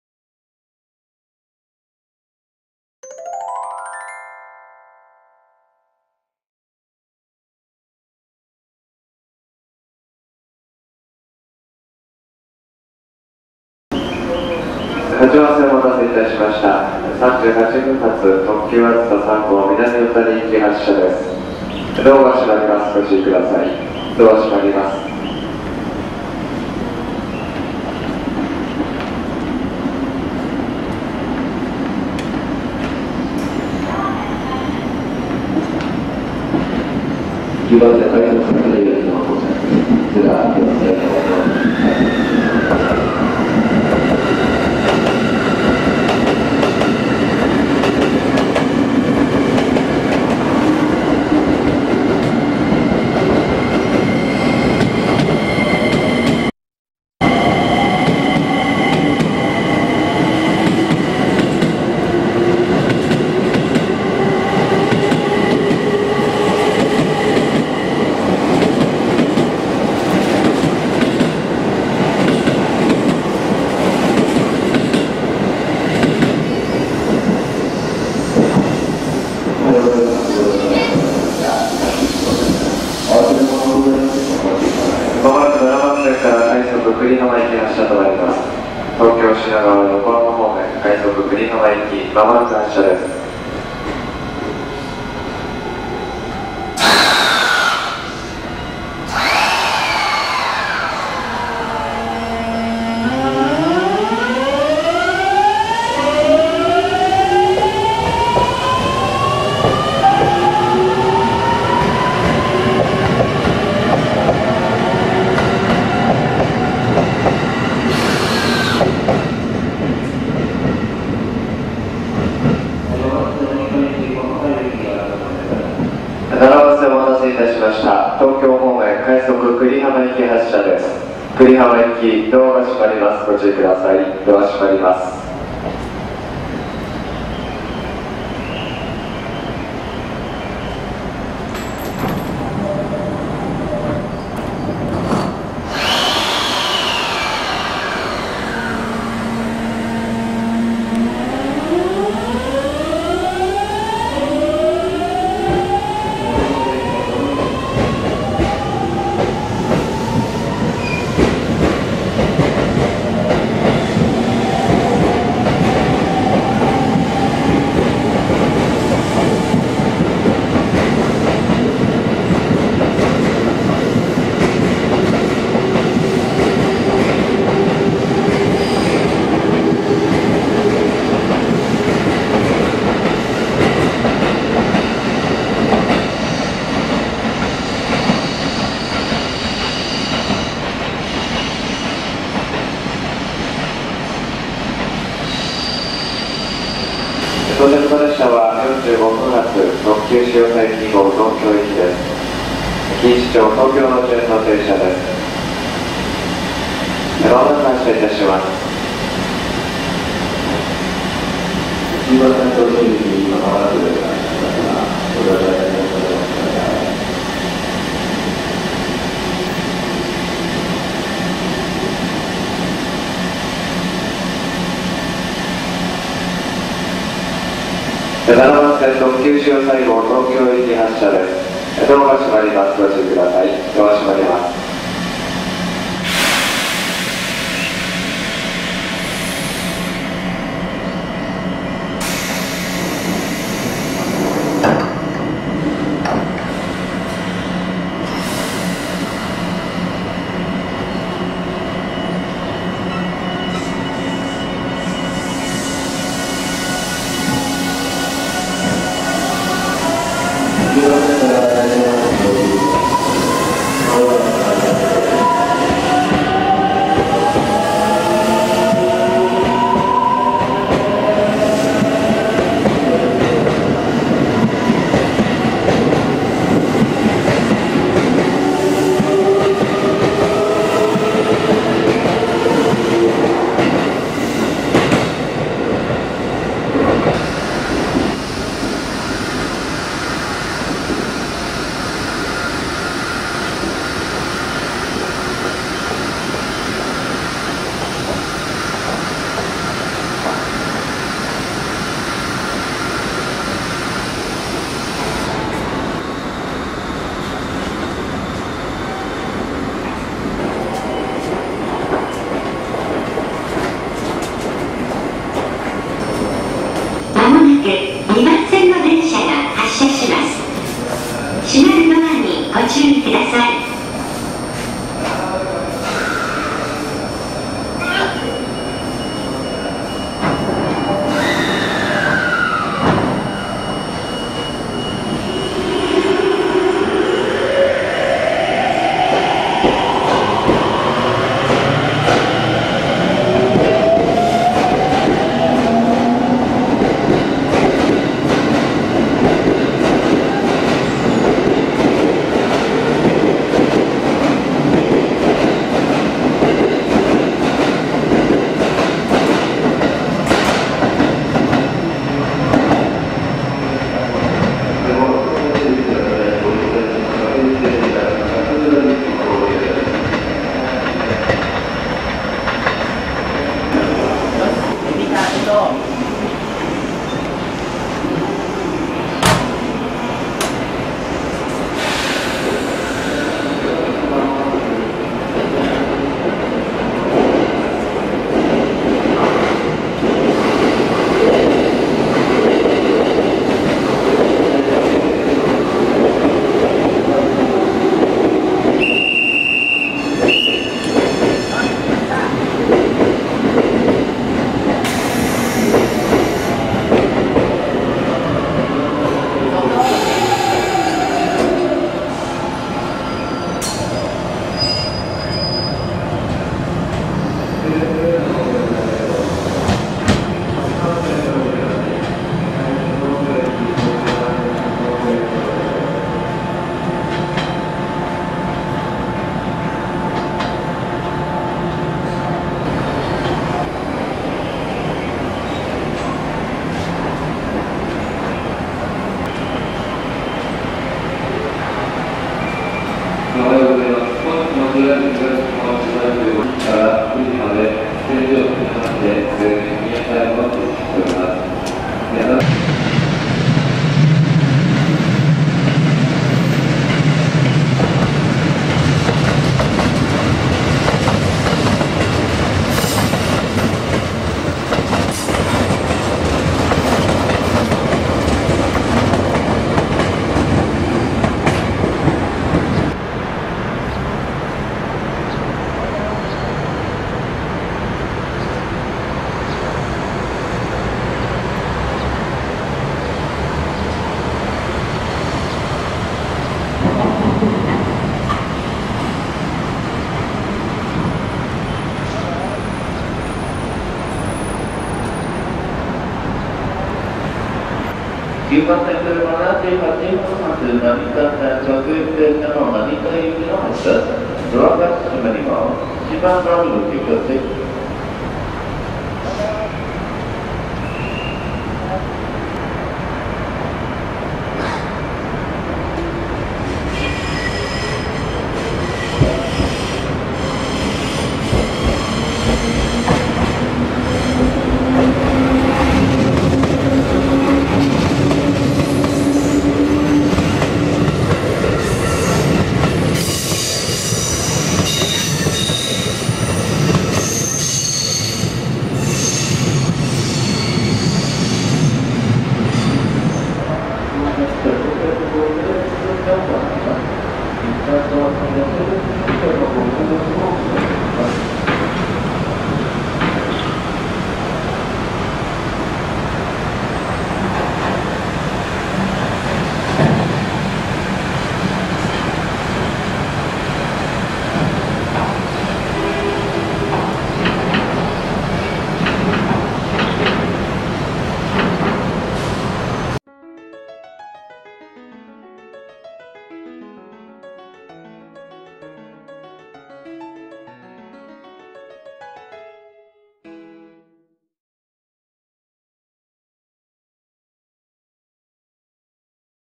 懐かしの千葉駅 2006 12
2006.12撮影
懐かしの千葉駅
257系あずさ3号　南小谷行き
113系.211系が登場します。
画質が良く有りません。ご了承ください。